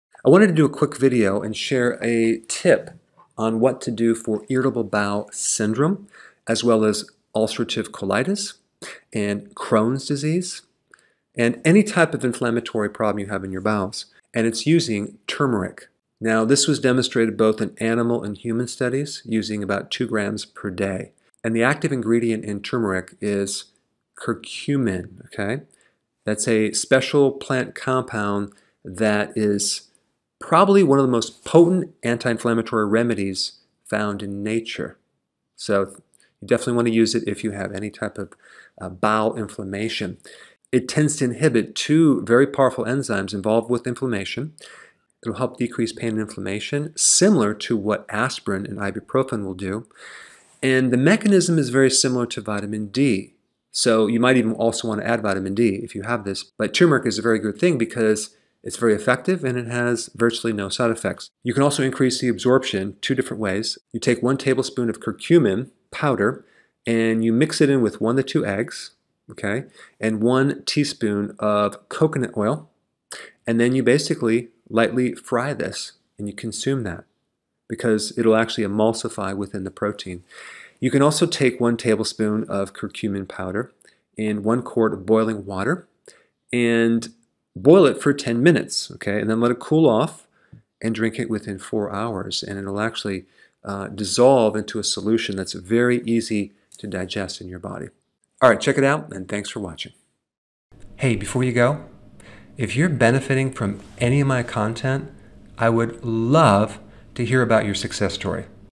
تناولوا الكركم بهذه الطريقة لتحصلوا على فوائده المذهلة!
سنتحدث عن علاج رائع لمتلازمة القولون العصبي والتهاب القولون التقرحي ومرض كرون وأنواع أخرى من حالات الالتهاب التي تؤثر على أمعائك.
العلاج هو الكركم. تظهر كل من الدراسات التي أجريت على الحيوانات والبشر أن تناول 2 جرام من الكركم يوميًا لعلاج متلازمة القولون العصبي يمكن أن يساعد في تقليل الالتهاب.
العنصر النشط في الكركم هو الكركمين. الكركمين هو مركب نباتي خاص يعمل كمضاد قوي للالتهابات. في الواقع ، الكركمين هو أحد أقوى مضادات الالتهاب الطبيعية في الطبيعة.
يمنع الكركمين اثنين من الإنزيمات المرتبطة بزيادة الالتهاب (COX2 و 5-LOX). كما أنه يقلل الألم بشكل مشابه للأسبرين والأيبوبروفين. يعمل الكركمين بشكل مشابه لفيتامين د.
أفضل جزء هو أن الكركمين طبيعي وليس له أي آثار جانبية.
يمكنك زيادة امتصاص الكركمين عن طريق تناول 1 ملعقة كبيرة من مسحوق الكركمين مع 1-2 بيضة و 1 ملعقة صغيرة من زيت جوز الهند.
يمكنك أيضًا محاولة إضافة 1 ملعقة كبيرة من الكركمين إلى 1 لتر من الماء المغلي لمدة 10 دقائق ثم تركه يبرد. اشرب الخليط في غضون أربع ساعات.
إذا وجدت هذا المحتوى مفيدًا ، فتأكد من مشاركة قصة نجاحك معنا! تحقق من الرابط أعلاه.
تابعنا على فيسبوك:ArabicDrBerg
وانستجرام: drberg.ar

شكراً للمشاهدة!